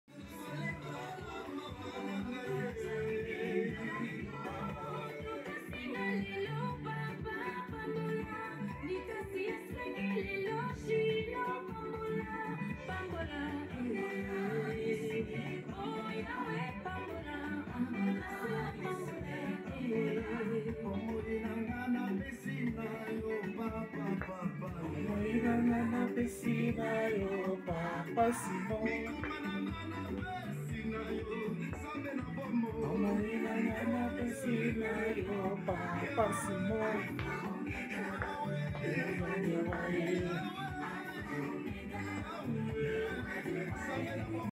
PARTI POLITIQUE ANGECRATE
Idéologie du Parti Politique Angécrate, mis en place par Sammy Nkangila Mbakiu Mangikila